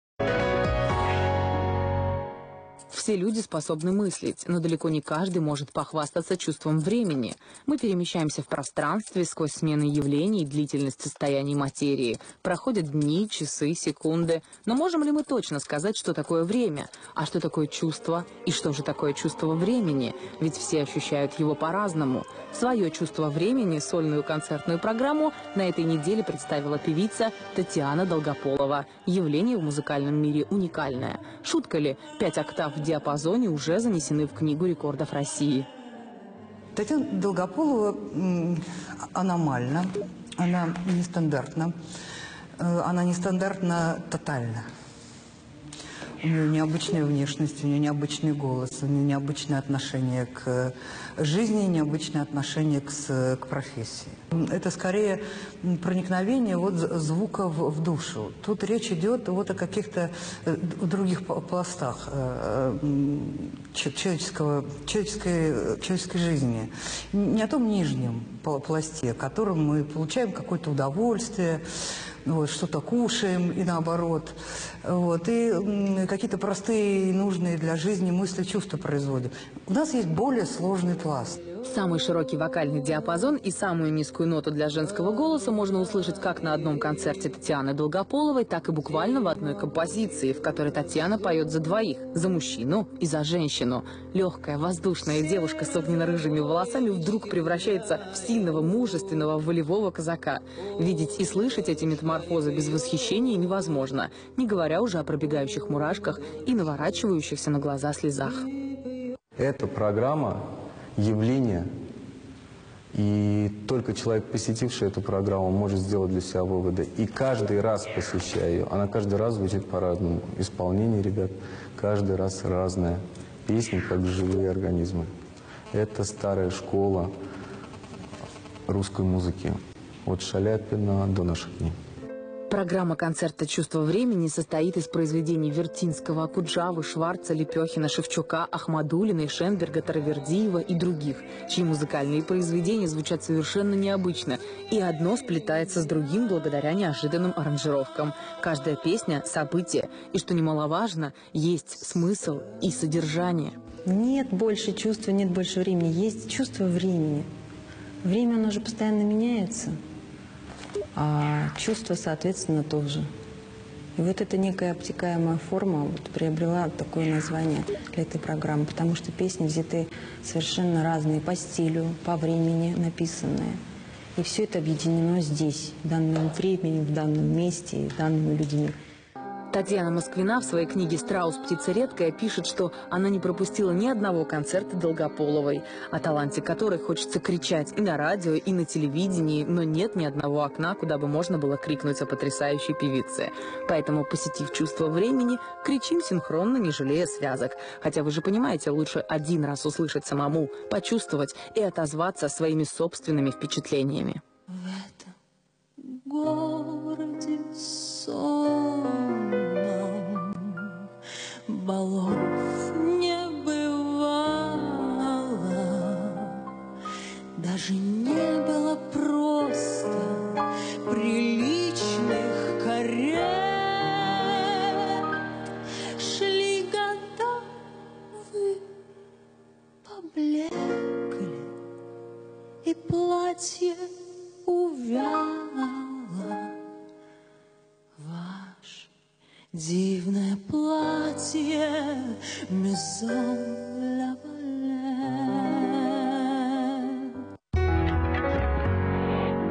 МУЗЫКАЛЬНЫЙ ТЕАТР ● ГОЛОС «ЧУВСТВО ВРЕМЕНИ»Татьяна Москвина о Татиане Долгополовой
Поддержка Театра: Сбер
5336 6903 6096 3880
Кирилл Богдатов.Спасибо!
Коллектив Музыкального театра «Голос»
Татиана Долгополова - вокал
Константин Поляков – рояль
Дмитрий Зарубин - гитара
Дмитрий Семенищев - контрабас
Главный Администратор
Ольга Георгиевна Студикова
Антон Нечипуренко - ХУДОЖНИК ПО СВЕТУ
Максим Стриженок - ВИДЕО-ОПЕРАТОР
Анастасия Филиппова -ФОТОГРАФ
Богдатов
Кирилл Николаевич - ДИРЕКТОР ТЕАТРА